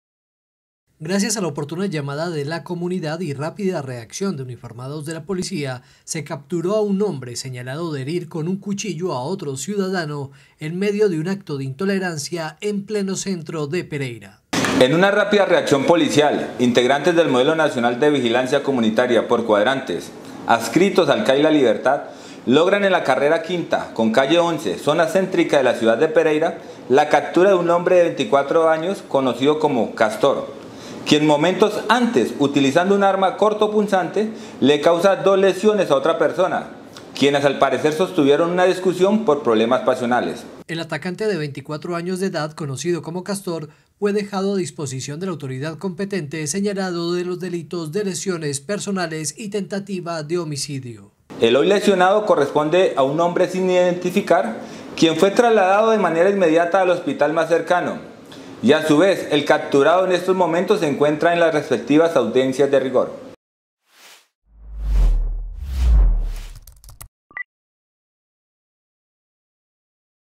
CAPTURADO ALIAS “CASTOR” EN CENTRO DE PEREIRA SEÑALADO DE LESIONAR CON ARMA BLANCA A OTRO HOMBRE
Gracias a la oportuna llamada de la comunidad y rápida reacción de los uniformados de la policía se pudo capturar a un hombre señalado de herir con un cuchillo a otro ciudadano en medio de un acto de intolerancia.